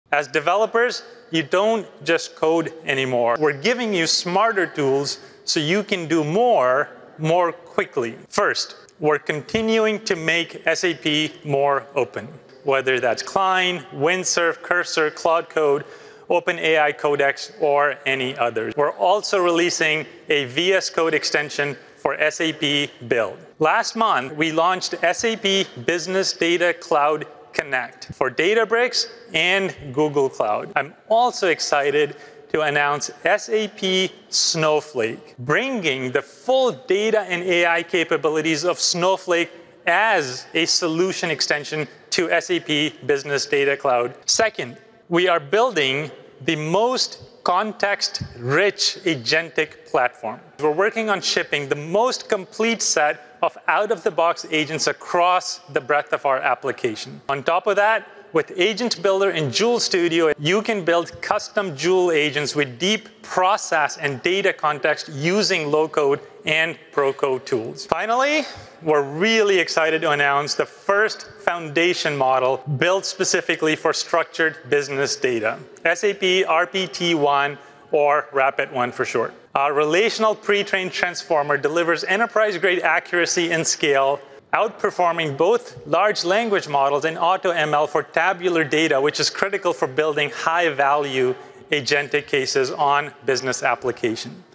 What We Announced at SAPTechEd 2025 | AI & Agents
This year, SAPTechEd put developers at the center of business AI innovation.

✅ Expanded tools in SAP Build for low-code development
✅ Partnership with Snowflake to power enterprise AI
✅ New learning resources to upskill 12M developers by 2030